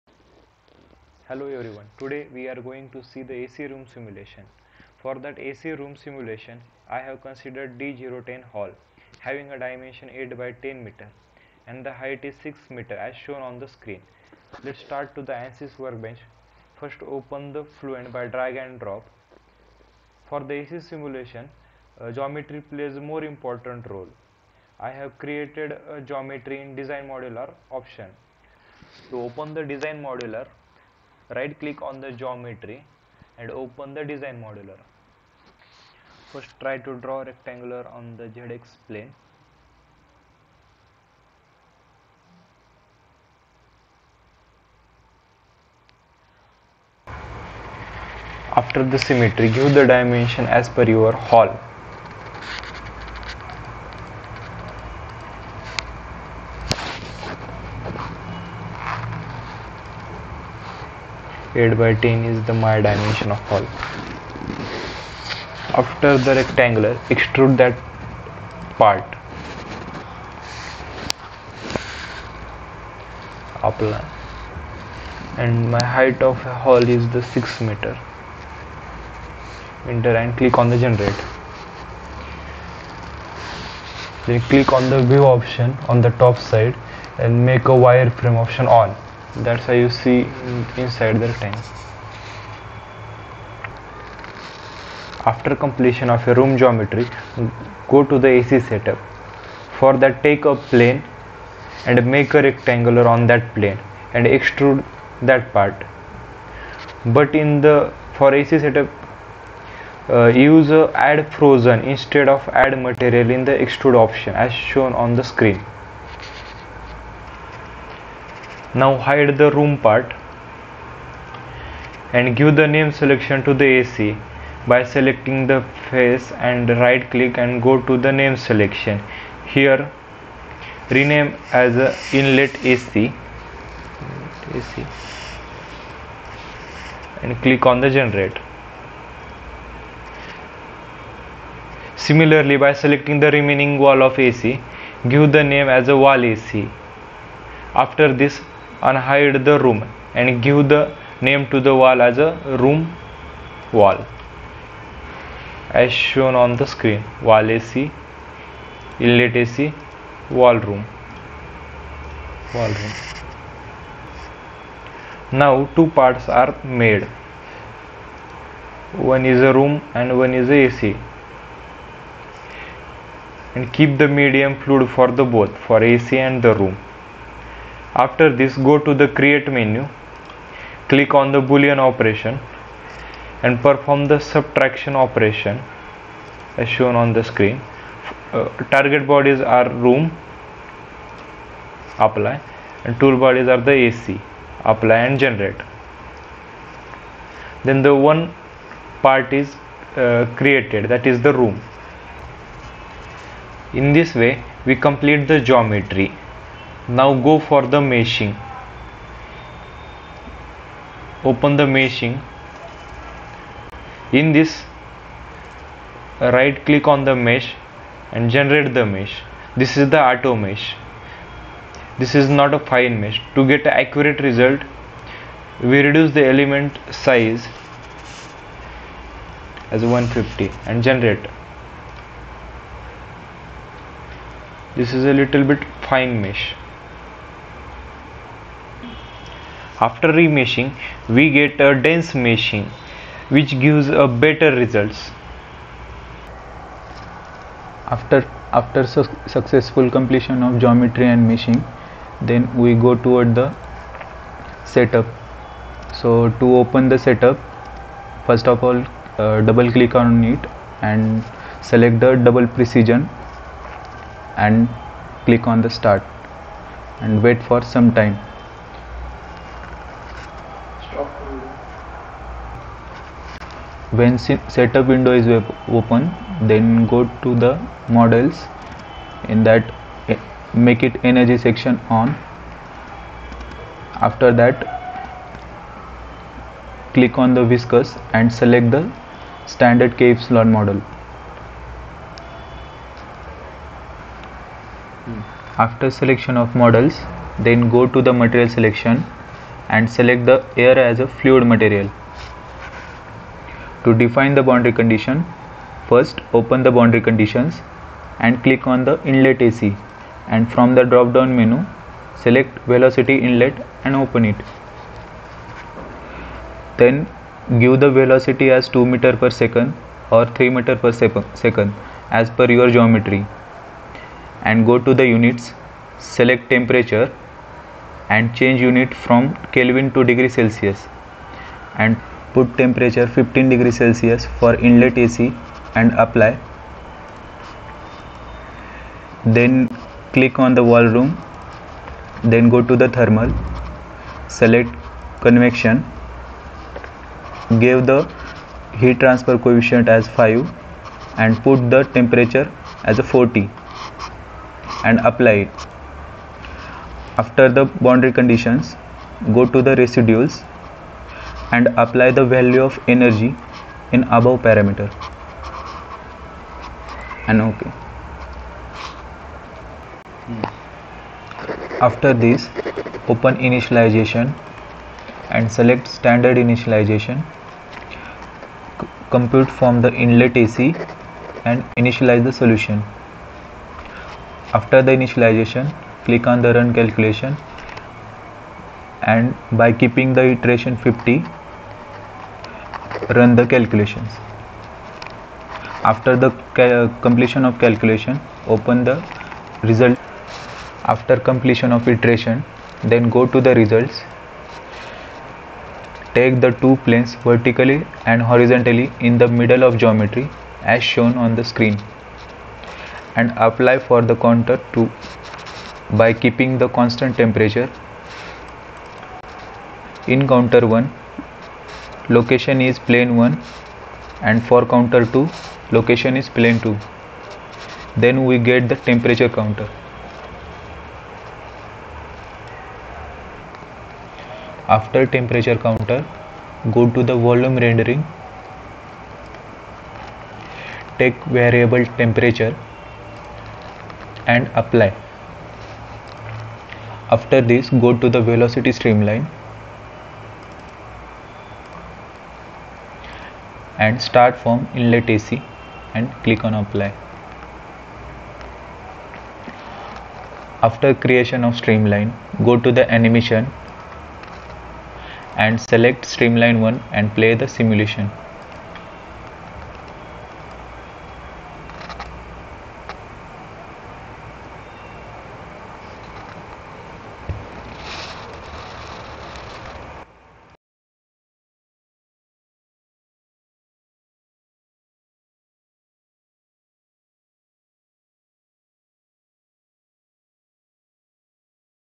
AC Room Simulation (CFD Analysis)
Simple AC room simulation is done by the CFD technique. ANSYS software is used to do the simulation